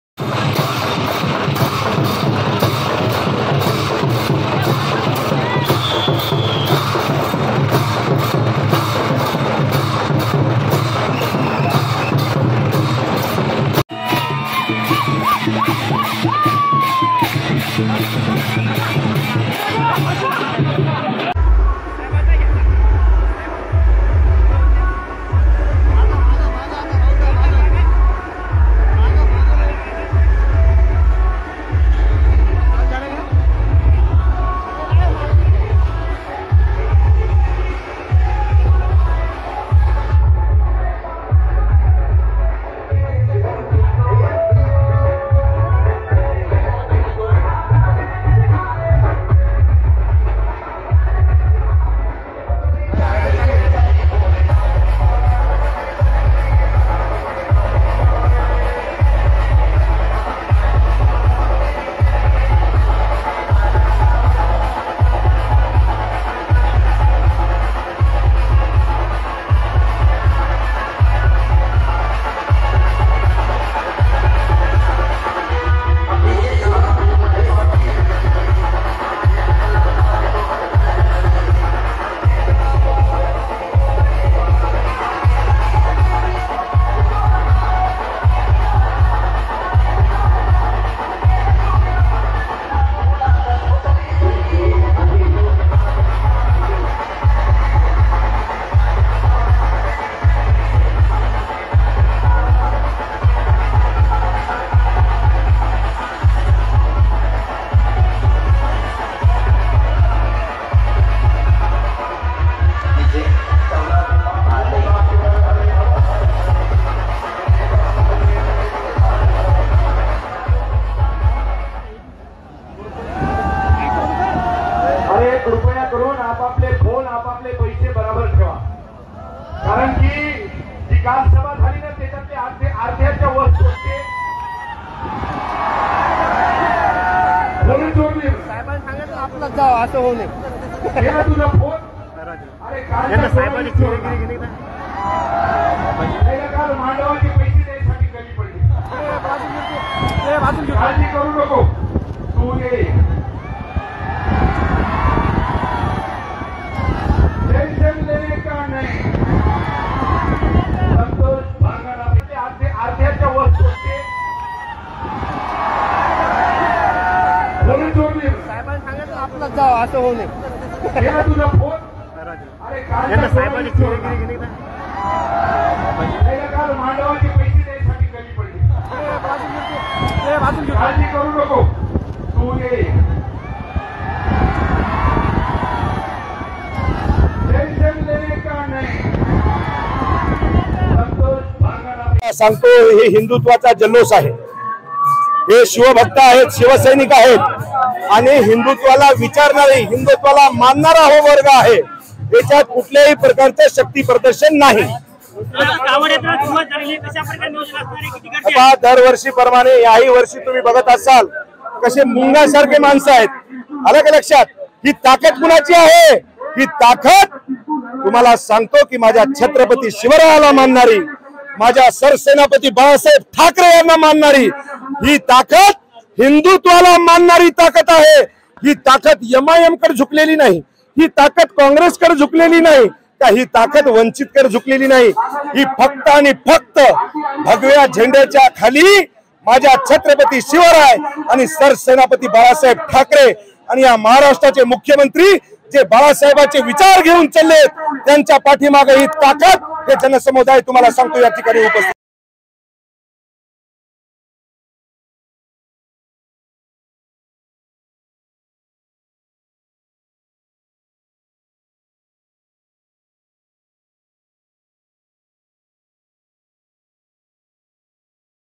Santosh Bangar | कावड यात्रेतून बांगरांनी दाखवली ताकद, म्हणाले... | LetsUpp Marathi
उद्धव ठाकरेंच्या सभेला उत्तर देण्यासाठी आमदार संतोष बांगर यांनी कावड यात्रेतून शक्ती प्रदर्शन केलं. त्यांच्या कावड यात्रेचा व्हिडिओ सोशल मीडियावर व्हायरल झाला आहे.